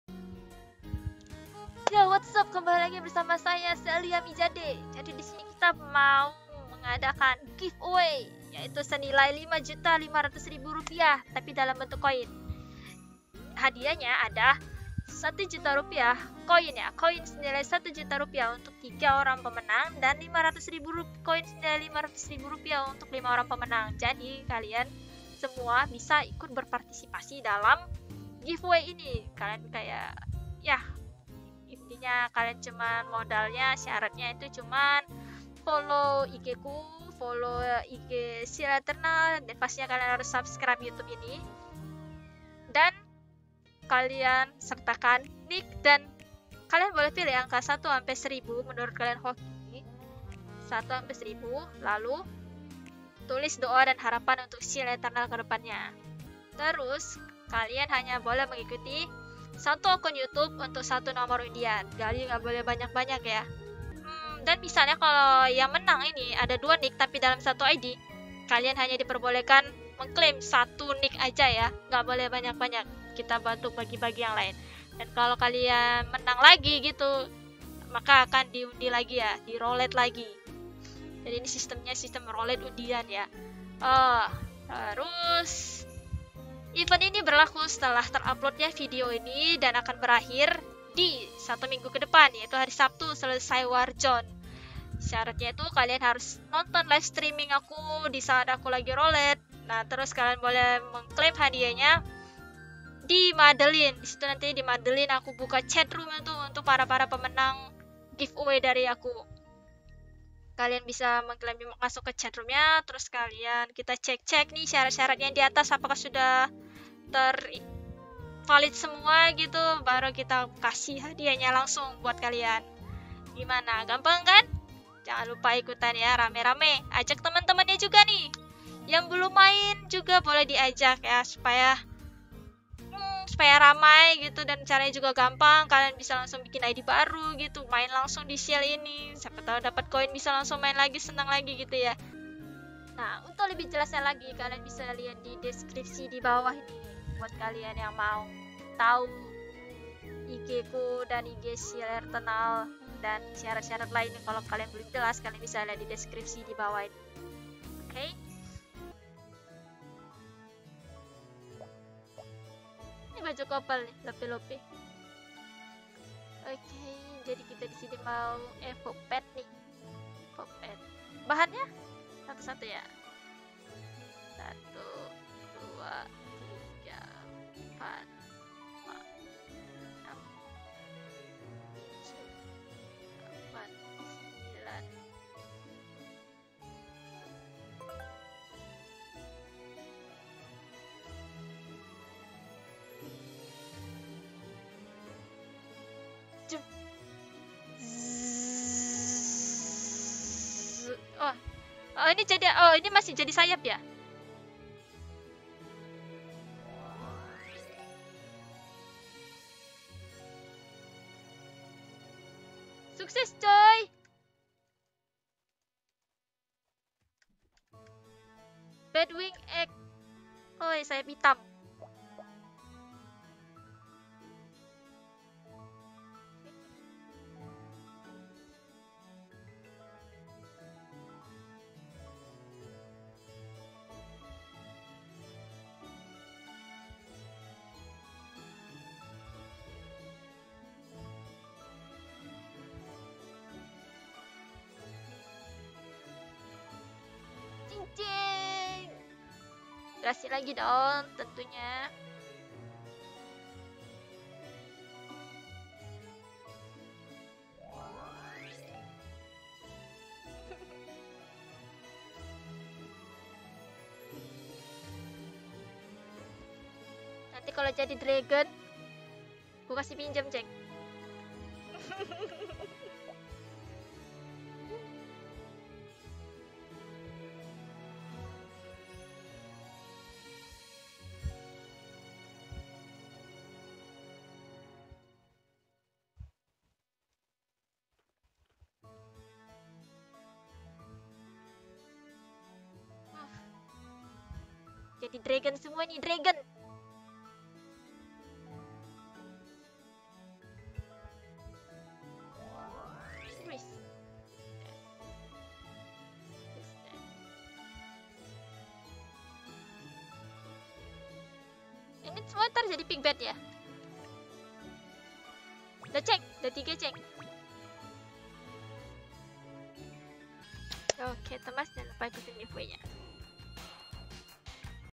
Seal Online Eternal Plus - Giveaway senilai Rp. 5.500.000 !!!
Rules :
- Follow Instagram @Sallyamizade dan @Seal.eternal
- Pilih dan tulis angka dari 1 - 1000  beserta IGN dan Diwajibkan untuk menulis doa terbaik untuk Seal Eternal di colom Commentar Youtube . apabila ada angka yang sama , maka pemenang akan diambil dari yang tercepat (comment pertama)
- Jika Pemenang tidak sesuai dengan rules di atas maka dianggap hangus dan diundi ulang
- Apabila terdapat pemenang dengan 2 IGN berbeda di 1 ID maka hanya 1 reward yg akan dikirim

contoh :
IGN : Jarwo , 284 . Semoga seal eternal makin jaya jaya jaya !!

Pendataan ditutup jam 5 sore ya guys ^^

Pengundian akan dilakukan tanggal 11 September 2021 dan tentunya akan ada Live streaming untuk pengundian tersebut

Reward :
Koin Masing" seharga 1 juta rupiah untuk 3 orang pemenang dan
Koin masing" seharga 500k rupiah untuk 5 orang pemenang ^^

Syarat Pengambilan Reward Give Away
- Diwajibkan untuk menonton livestreaming dan berkumpul di Madelin .

Page Fb : @sallyicedeath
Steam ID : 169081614